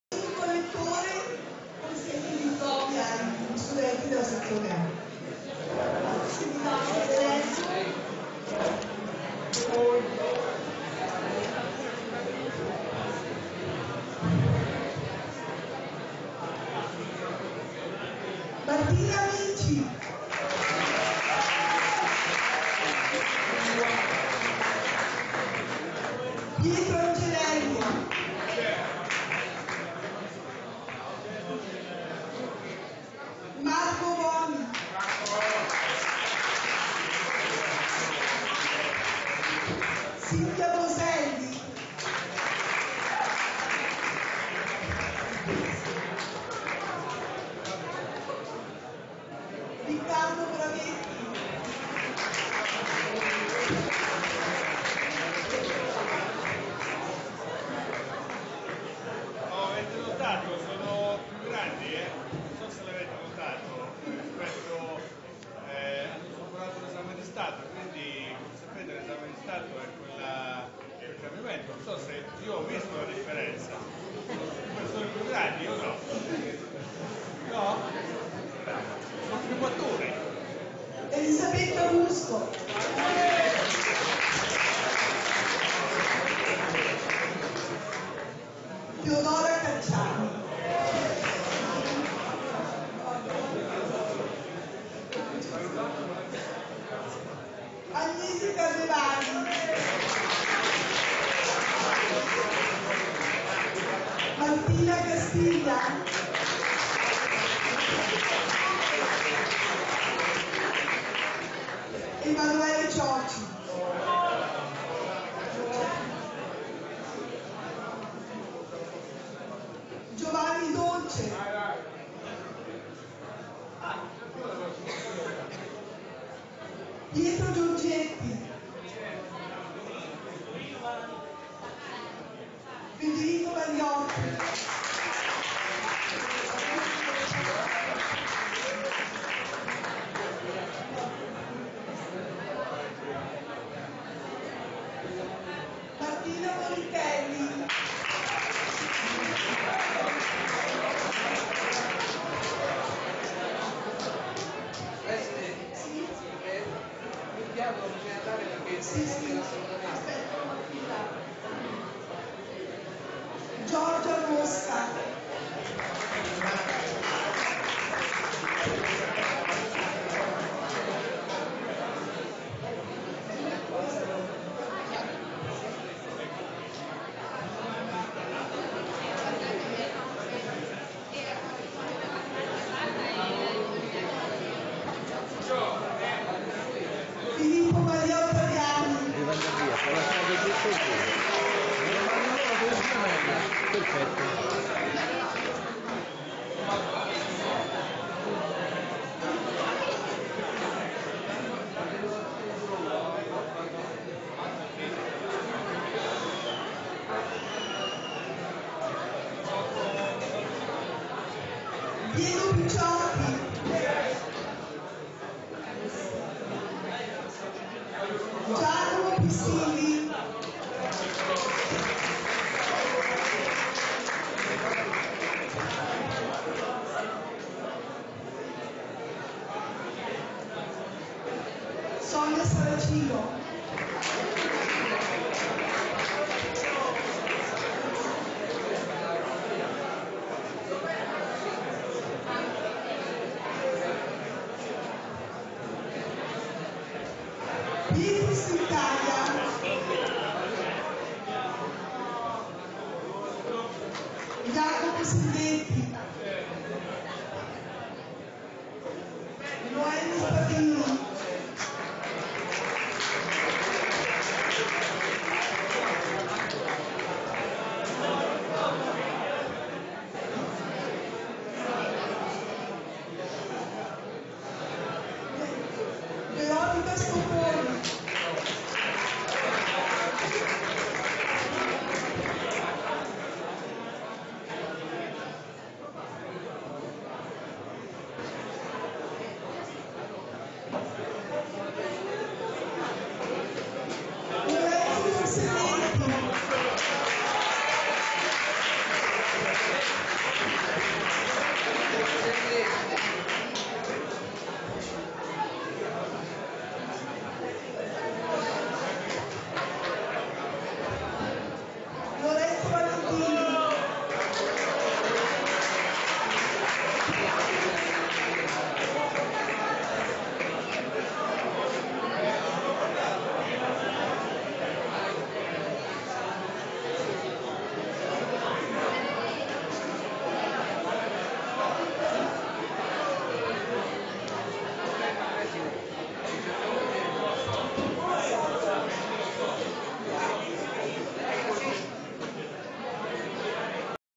Il Magnifico Rettore Longhi consegna i diplomi al VA Fabriano
Cerimonia di consegna dei diplomi 2013